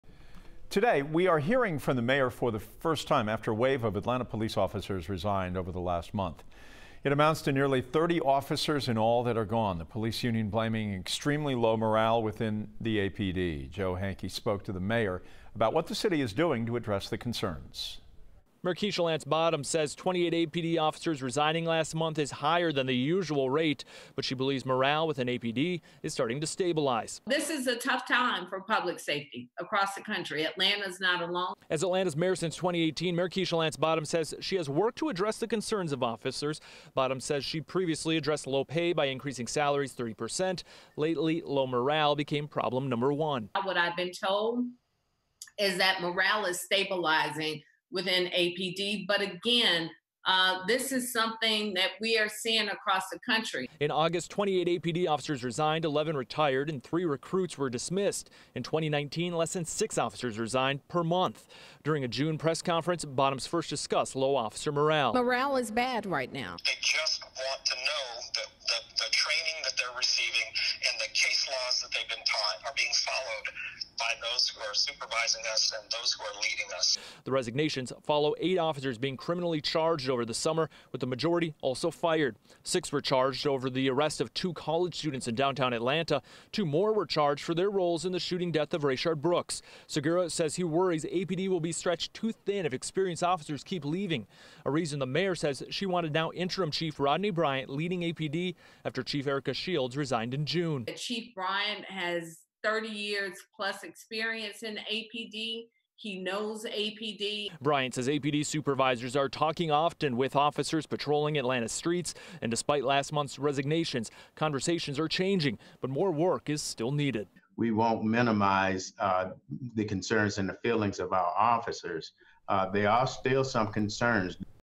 Mayor: Atlanta Police seeing 'higher than normal resignation rate' but low morale is stabilizing
Atlanta Mayor Keisha Lance Bottoms addressed the rising rate of police resignations in a press briefing on Wednesday,